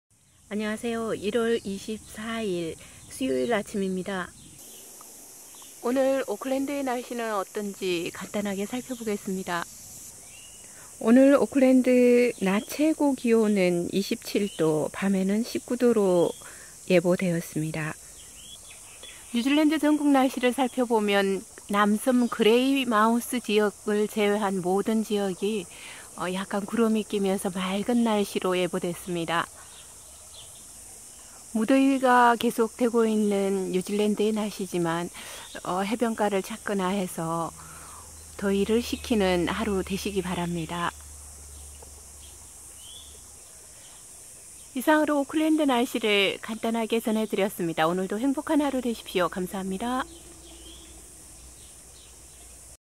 1월 24일, 오클랜드는 뜨거운 햇살 가득
1월 24일 오클랜드는 강한 햇살이 대지를 데우는 날씨가 될 것으로 보입니다.

낮 최고 기온 27도, 밤에는 19도가 될 것이라 예보되었습니다.

뉴질랜드 전국 날씨는 남섬 그레이마우스에서만 약간의 소나기 소식이 있고 그 외 지역은 여름햇살이 강렬한 하루가 될 것으로 예상됩니다.​